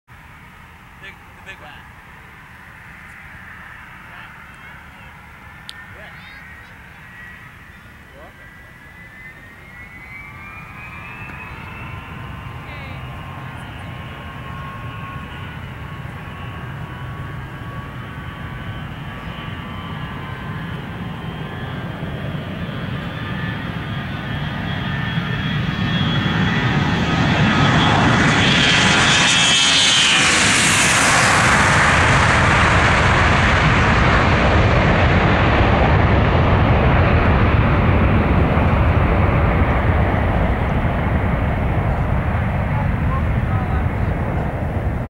Gulfstream V [N6458] Takeoff Madras Airport (S33)
Flight: ??? S33-HIO
Date: August 21, 2017
Taking off from runway 34